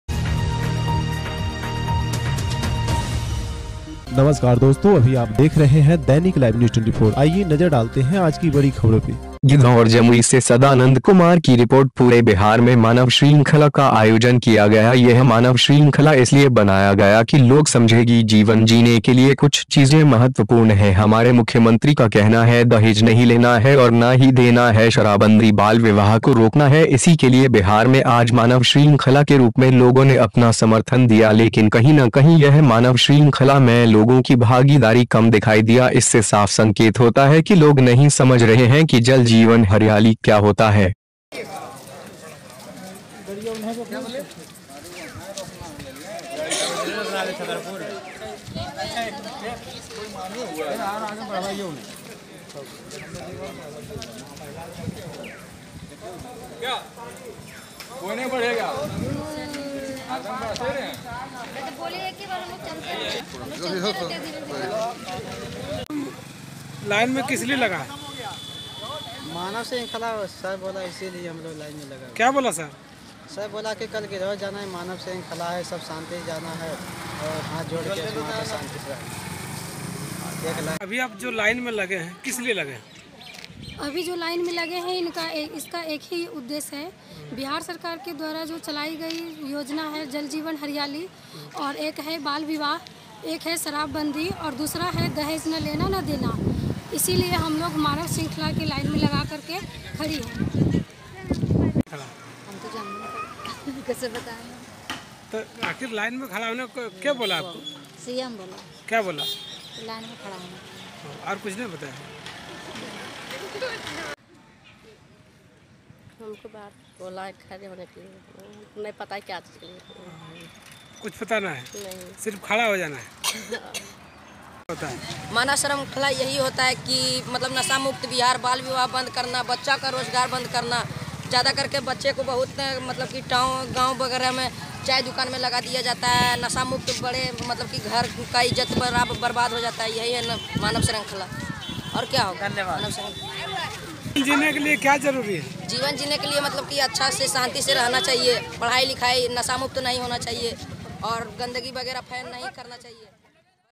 मानव श्रृंखला इसलिए बनाया गया कि लोग समझेगी जीवन जीने के लिए कुछ चीजें महत्वपूर्ण है
गिधौर जमुई से सदानन्द कुमार की रिपोर्ट

पूरे बिहार में मानव श्रृंखला का आयोजन किया गया यह मानव श्रृंखला इसलिए बनाया गया कि लोग समझेगी जीवन जीने के लिए कुछ चीजें महत्वपूर्ण है हमारे मुख्यमंत्री का कहना है दहेज नहीं लेना है और ना ही देना है शराबबंदी बाल विवाह को रोकना है इसी के लिए बिहार में आज मानव श्रृंखला के रूप में लोगों ने अपना समर्थन दिया लेकिन कहीं ना कहीं यह मानव श्रृंखला मैं लोगों की भागीदारी कम दिखाई दिया इससे साफ संकेत होता है की लोग नहीं समझ रहे हैं कि जल जीवन हरियाली क्या होता है